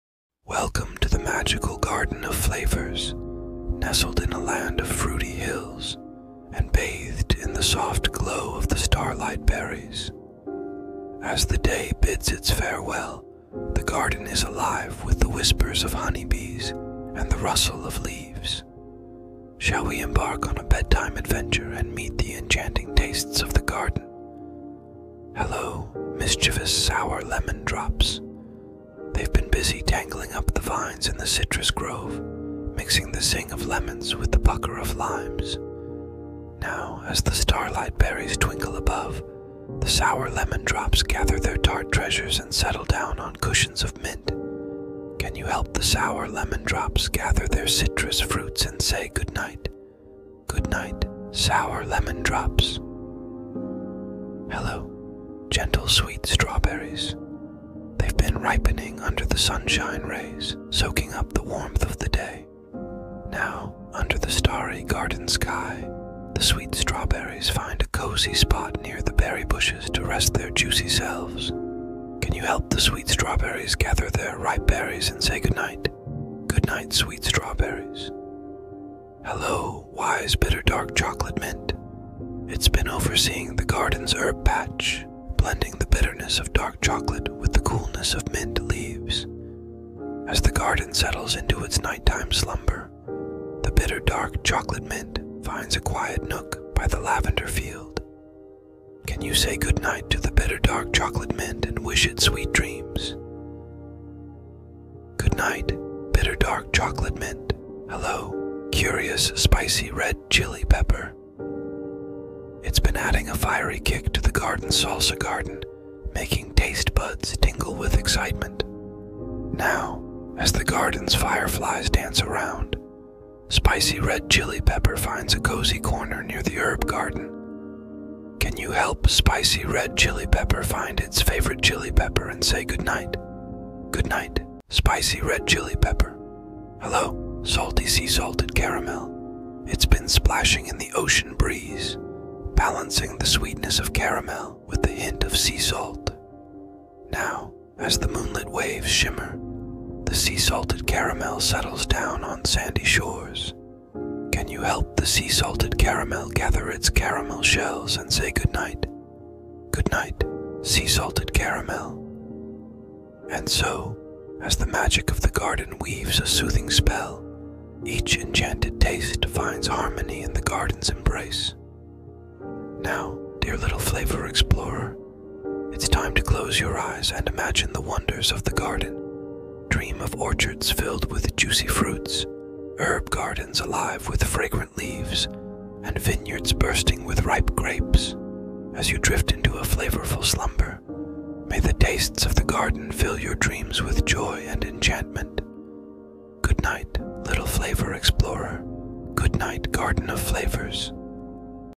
Goodnight tastes | Goodnight the World
Introduce kids to various tastes: sour, sweet, bitter, spicy, salty.